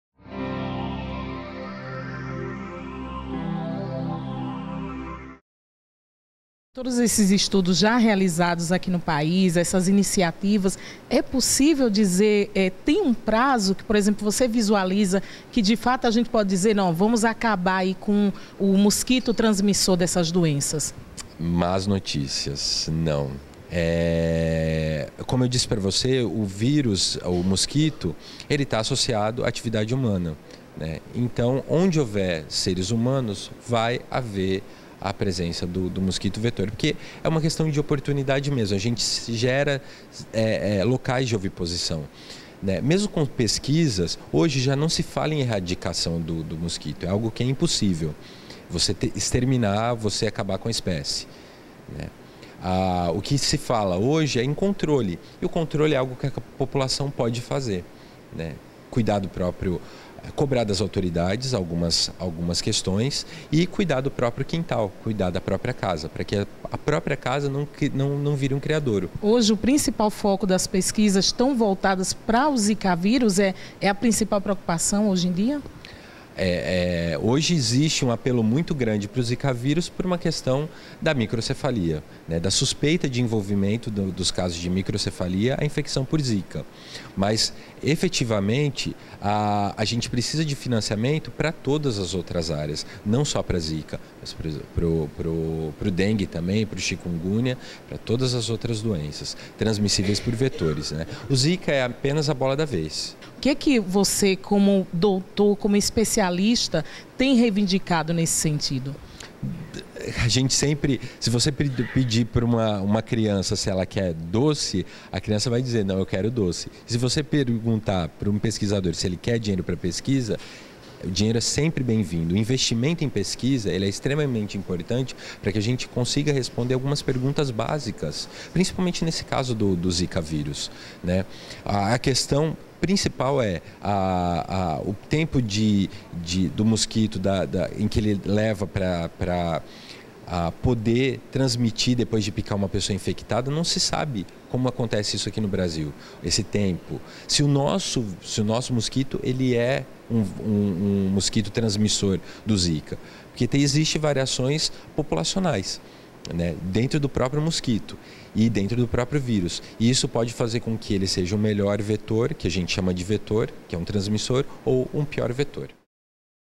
Especialista diz que Aedes aegypti não será extinto
Dengue, zika e chikungunya os vírus causados pelo mosquito aedes aegypti tem preocupado os brasileiros. Com a relação entre o zika vírus e o nascimento de crianças com a microcefalia a população passou a ficar mais atenta as causas e a proliferação do mosquito transmissor. Para responder a perguntas sobre a relação entre o mosquito e as doenças a equipe do Portal Infonet ouviu o Drº Adriano Mondini que é especialista em doenças transmitidas pelo mosquito aedes aegypti